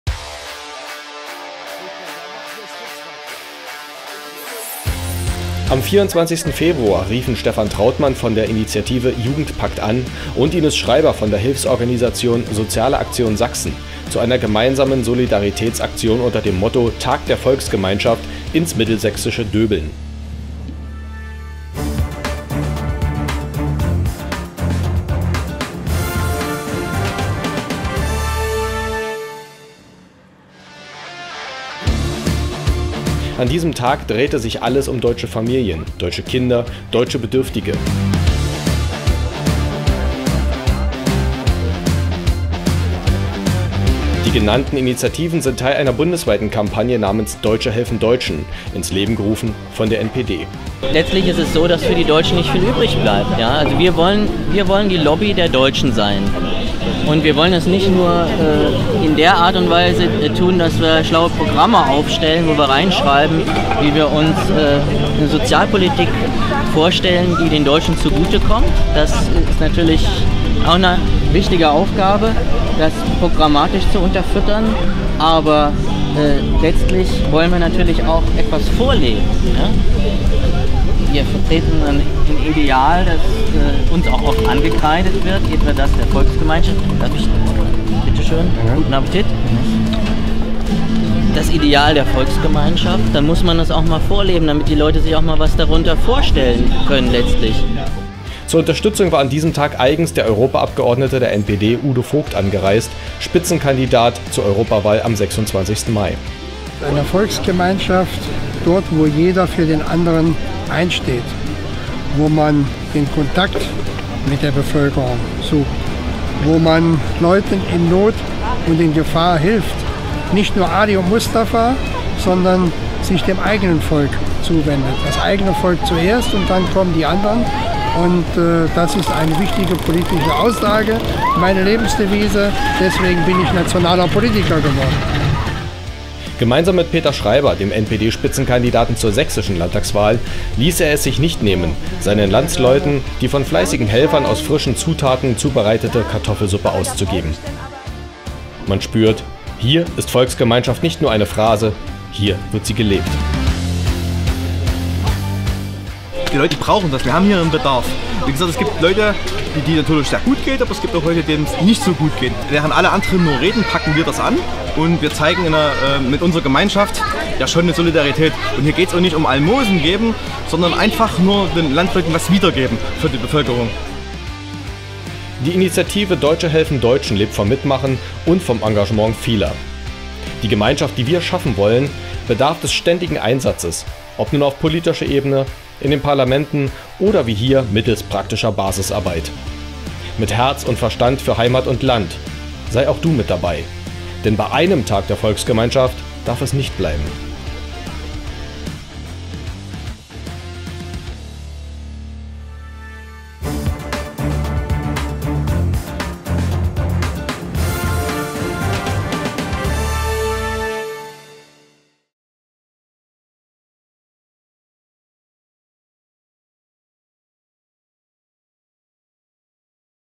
DS-TV 3-19 Volksgemeinschaft - DEUTSCHE HELFEN DEUTSCHEN macht vor
Volksgemeinschaft - DEUTSCHE HELFEN DEUTSCHEN macht vor, wie´s geht:

"Anpacken statt zuschauen" ist die Devise der NPD. Denn für alle und jeden gibt es eine Lobby in diesem Land - nur nicht für die Einheimischen.

Deshalb belässt es die NPD nicht nur bei politischen Forderungen, sondern setzt diese auch gleich in die Praxis um. Jetzt einbringen und mitmachen!